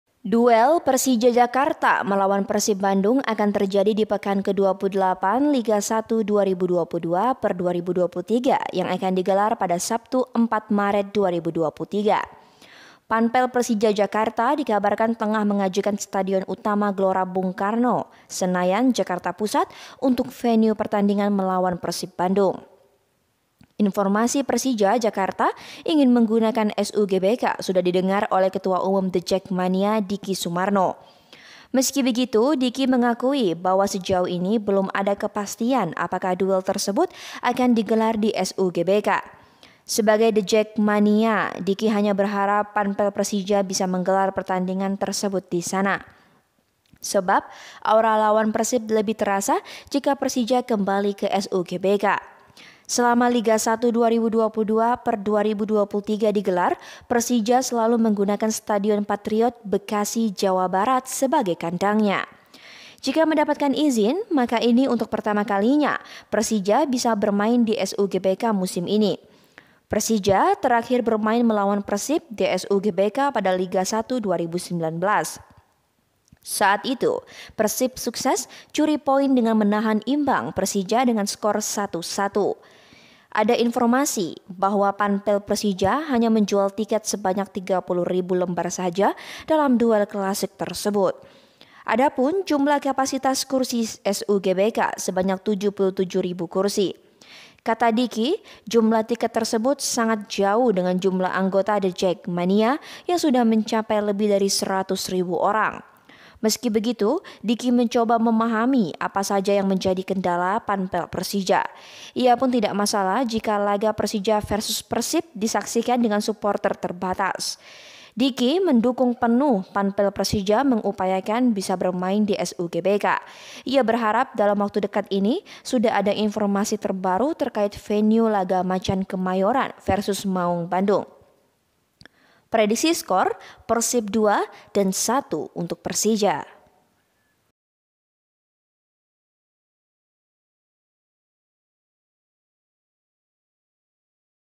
🔴 Prediksi Skor Persija Jakarta vs Persib Bandung, Panpel Siapkan 30 Ribu Tiket untuk Suporter
Duel Persija Jakarta melawan Persib Bandung akan tersajdi di pekan ke-28 Liga 1 2022/2023 yang akan digelar pada Sabtu (4/3/2023).

Panpel Persija Jakarta dikabarkan tengah mengajukan Stadion Utama Gelora Bung Karno (SUGBK), Senayan, Jakarta Pusat, untuk venue pertandingan melawan Persib Bandung.

Informasi Persija Jakarta ingin menggunakan SUGBK sudah didengar oleh Ketua Umum The Jakmania, Diky Soemarno.

Meski begitu, Diky mengakui bahwa sejauh ini belum ada kepastian apakah duel tersebut digelar di SUGBK.

Sebagai The Jakmania, Diky hanya berharap Panpel Persija bisa menggelar pertandingan tersebut di sana.

Sebab, aura lawan Persib lebih terasa jika Persija kembali ke SUGBK.

Selama Liga 1 2022/2023 digelar, Persija selalu menggunakan Stadion Patriot, Bekasi, Jawa Barat, sebagai kandangnya.

Jika mendapatkan izin, maka ini untuk pertama kalinya Persija bisa bermain di SUGBK musim ini.

Persija terakhir bermain melawan Persib di SUGBK pada Liga 1 2019.

Saat itu, Persib sukses curi poin dengan menahan imbang Persija dengan skor 1-1.

Ada informasi bahwa Panpel Persija hanya menjual tiket sebanyak 30 ribu lembar saja dalam duel klasik tersebut.

Adapun jumlah kapasitas kursi SUGBK sebanyak 77 ribu kursi.

Kata Diky, jumlah tiket tersebut sangat jauh dengan jumlah anggota The Jakmania yang sudah mencapai lebih dari 100 ribu orang.

Meski begitu, Diky mencoba memahami apa yang menjadi kendala Panpel Persija.

Ia pun tidak masalah jika laga Persija Vs Persib disaksikan dengan suporter terbatas.

Diky mendukung penuh Panpel Persija mengupayakan bisa bermain di SUGBK.

Ia berharap dalam waktu dekat ini sudah ada informasi terbaru terkait venue laga Macan Kemayoran Vs Maung Bandung.

Prediksi Skor
Persib 2 - 1 Persija